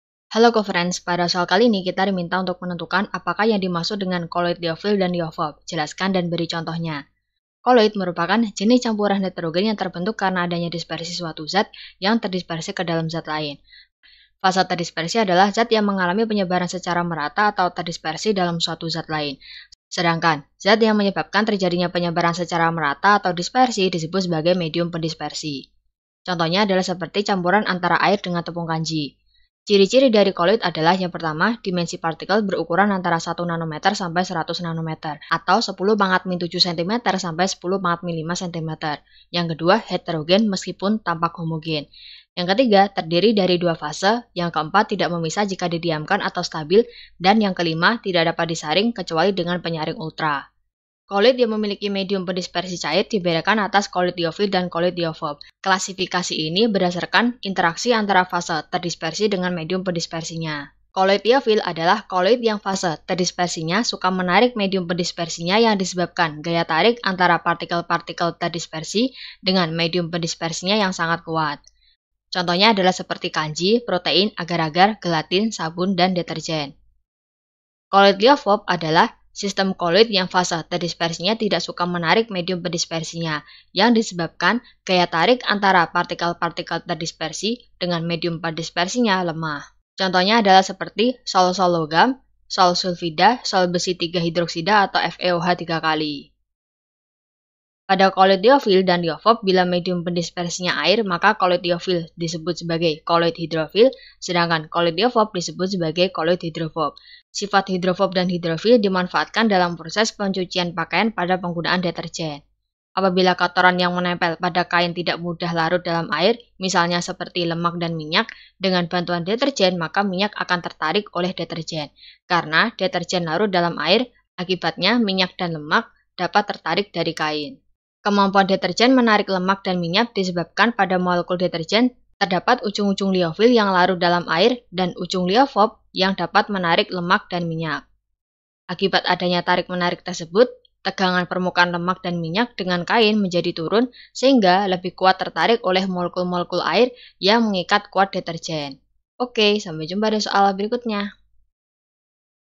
Apakah yang dimaksud dengan koloid liofil dan liofob? Jelaskan dan beri contohnya!
Topik atau Materi: Sifat Koloid - Sistem Koloid - Kimia Fisik dan Analisis - Kimia Kelas 11 SMA - KTSP

Sumber Soal: Bank Soal Kimia SMA, KTSP, SND 2009 , N/A

00:00 : Bedah Soal
01:00 : Konsep, rumus dan pengertian Sifat Koloid
01:18 : Langkah penyelesaian soal
01:32 : Jawaban akhir
02:02 : Penutup